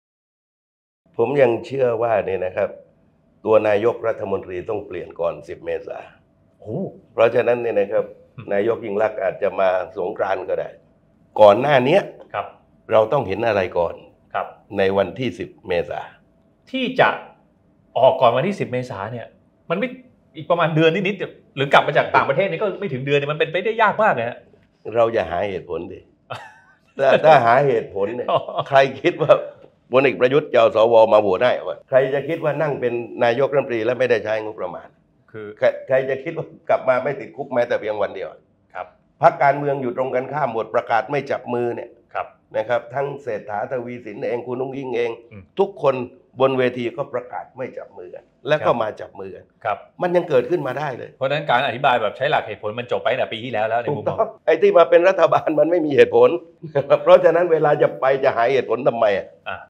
จตุพร ฟันฉับ ‘เศรษฐา’ จะเป็นนายกฯ อยู่ไม่ครบเทอม และฟังชัดๆ จะหลุดตำแหน่งช่วงกลางเดือนเมษายนปีนี้
จตุพร พรหมพันธุ์ แกนนำคณะหลอมรวมประชาชน ฟันฉับ ‘เศรษฐา’ จะเป็นนายกฯ อยู่ไม่ครบเทอม และฟังชัดๆ จะหลุดตำแหน่งช่วงกลางเดือนเมษายนปีนี้ และมีผลต่อการกลับมาของอดีตนายกฯ ‘ยิ่งลักษณ์’

ไทยรัฐทีวี ช่อง 32 และไทยรัฐออนไลน์